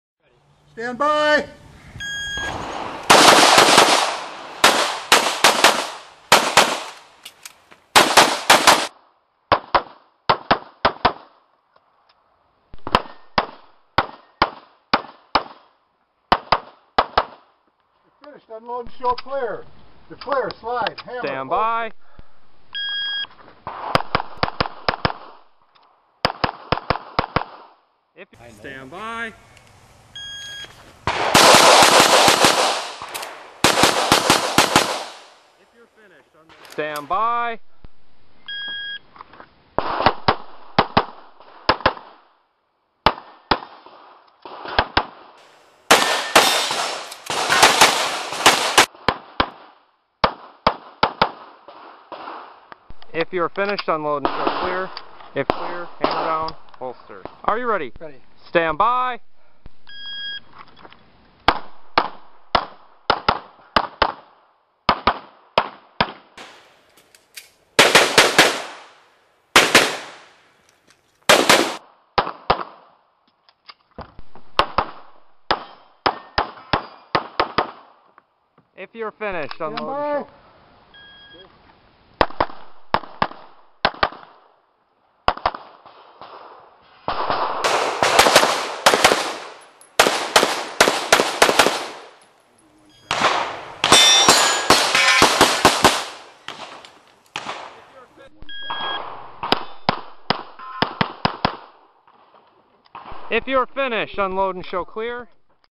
Ben Holmen June 2010
Ben Stoeger at the June 2010 Holmen USPSA local Match.